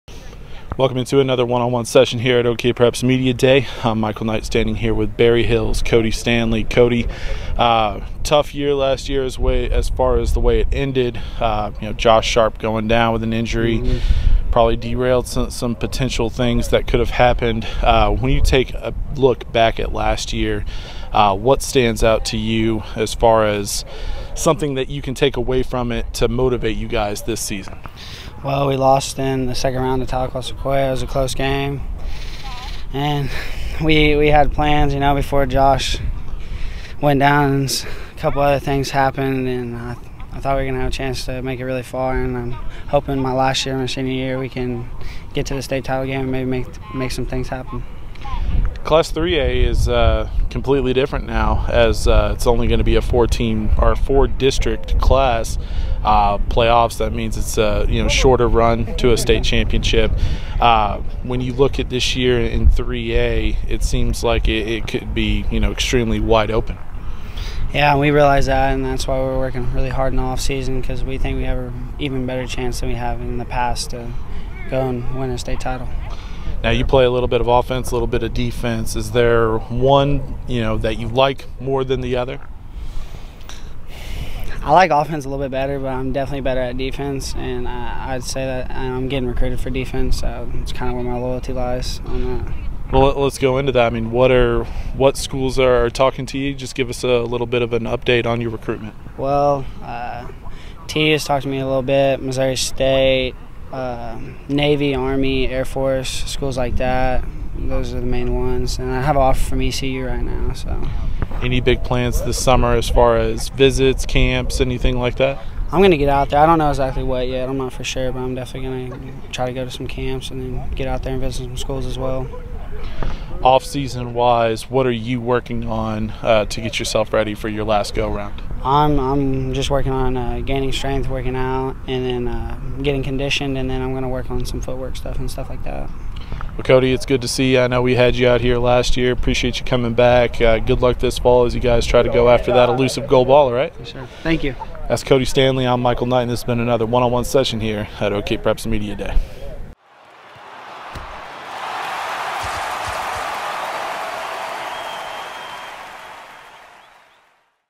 2018 Media Day: Kody Stanley
Check out the OKPreps Media Day interview with Berryhill tight end and defensive end Kody Stanley.